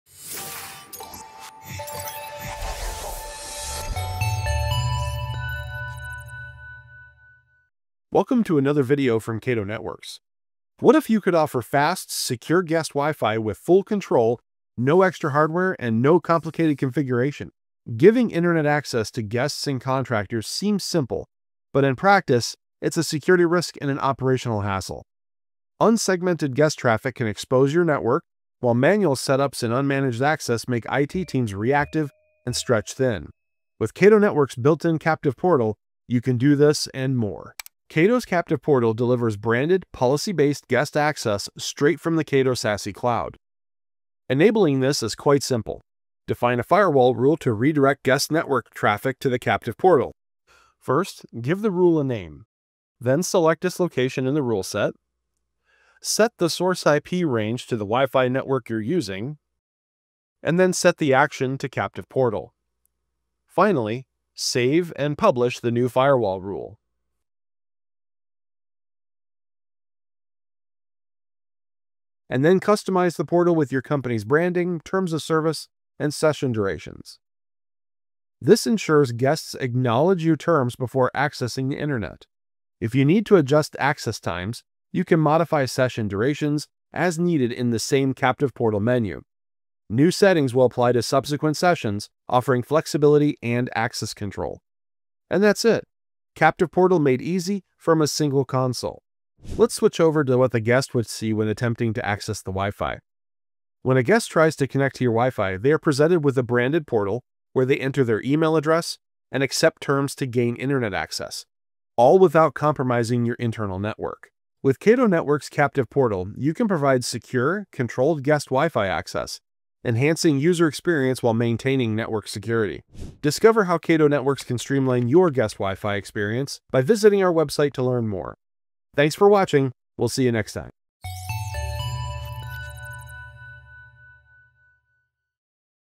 From Zero to Guest WiFi in Minutes with Cato Captive Portal
See how the Cato Captive Portal makes guest WiFi access simple and secure. In this demo, we show:
• The use case: Guest WiFi access without added appliances or complexity
• The value: Centralized visibility, security enforcement, and user control
• The proof: Quick deployment through the Cato Management Application
With Cato, IT teams can enable secure guest WiFi in minutes—no extra hardware, no siloed solutions.

Introduction 00:00
Firewall Rule 00:38
Captive Portal Settings 01:12
Captive Portal In Action 01:40
Conclusion 01:54